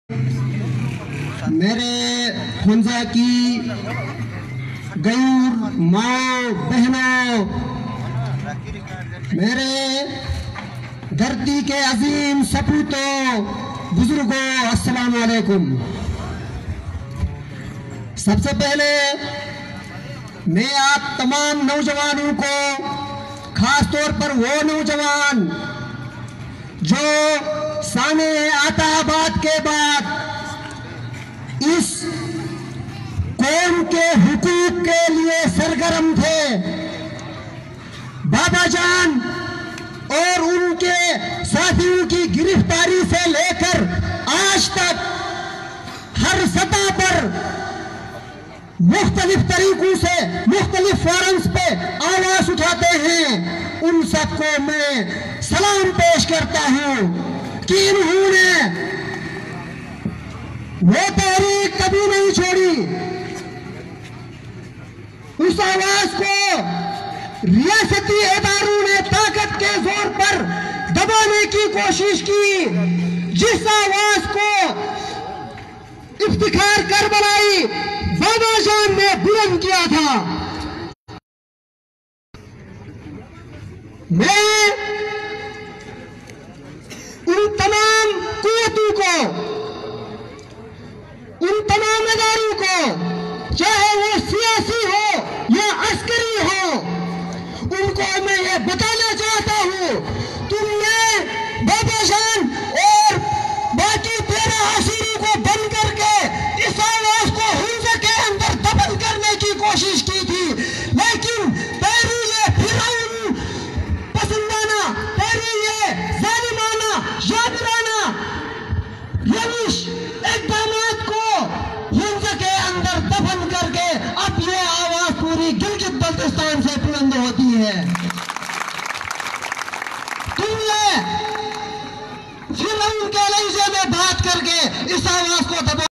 ilyas hunzai|Advocate M Taqi is addressing in Hunza
Free Baba Jan .advocate M Taqi is addressing the gathering in Hunza .